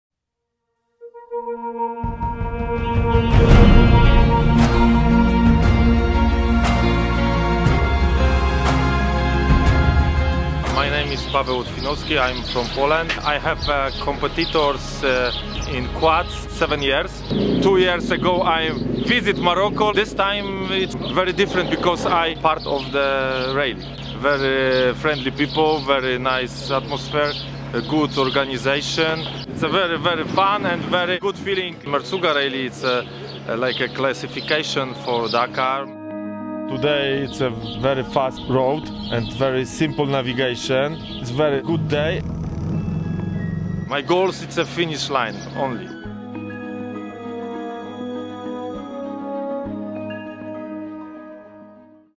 AFRIQUIA MERZOUGA RALLY - STAGE 4 - MERZOUGA HEROES
See the heroes of the 2018 edition of the Afriquia Merzouga Rally.